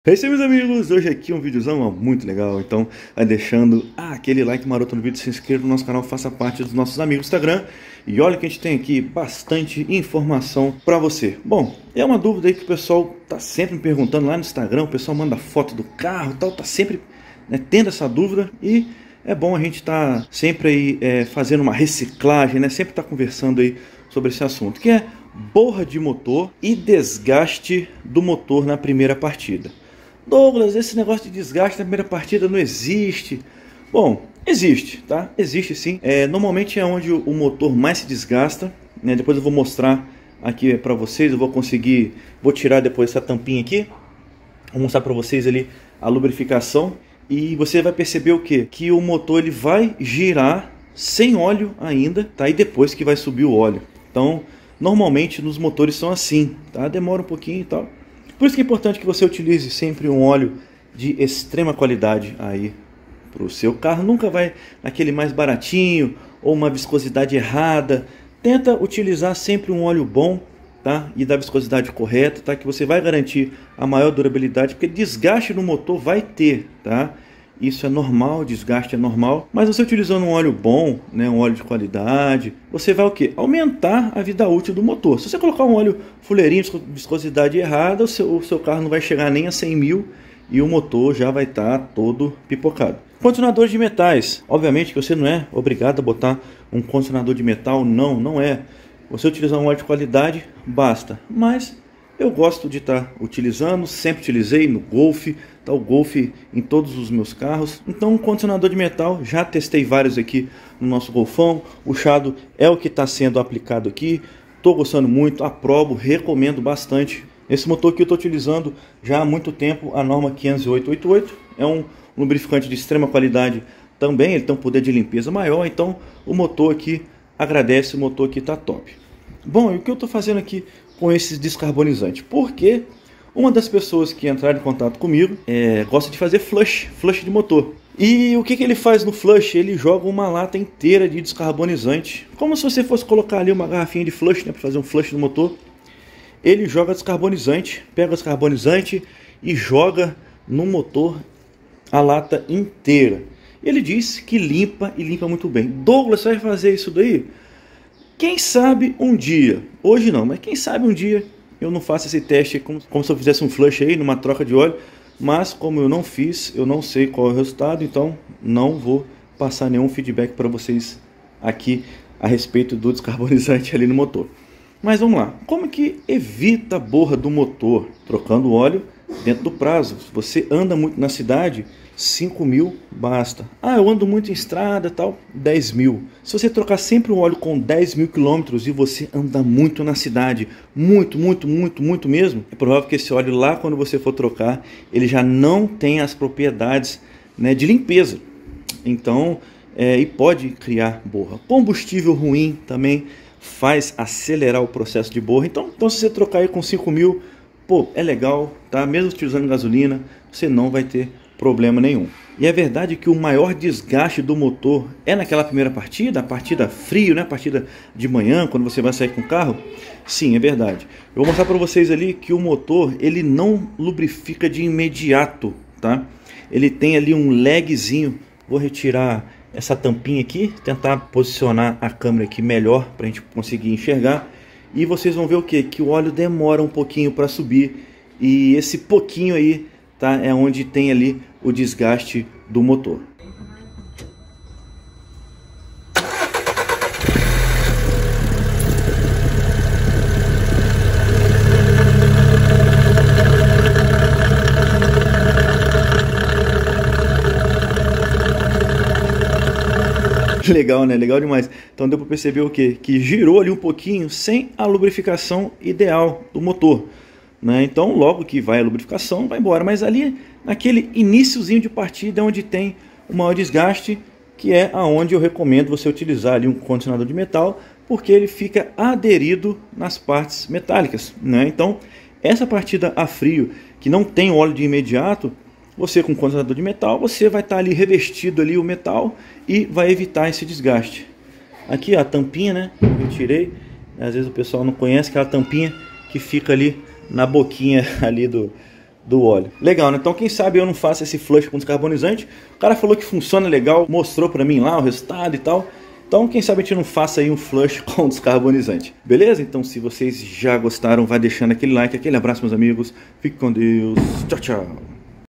EU APRENDI ESSA AGORA! MOTOR MACIO SEM BORRA! ELIMINE O DESGASTE NA PRIMEIRA PARTIDA DO CARRO!
🚀PARCERIAS, TESTE DE PRODUTOS.
Entre em contato conosco!

NESTE VÍDEO VOU MOSTRAR PARA VOCÊS COMO EVITAR BORRA DE ÓLEO NO MOTOR, E COMO REDUZIR DESGASTES NA PRIMEIRA PARTIDA, SENDO QUE A LUBRIFICAÇÃO DEMORA ALGUNS SEGUNDOS! MELHORE O MOTOR DO SEU CARRO! AUMENTE A VIDA ÚTIL DOS COMPONENTES!